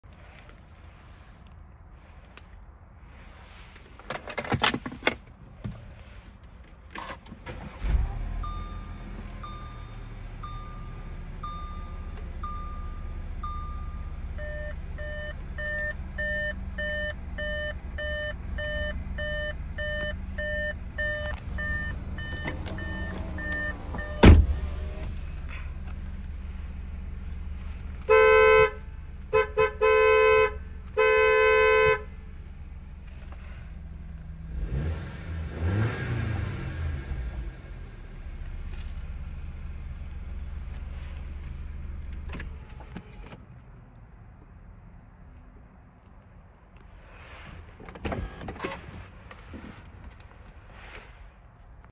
2013 Audi A5 2.0T Premium Plus Convertible startup and horn
0:27 is the horn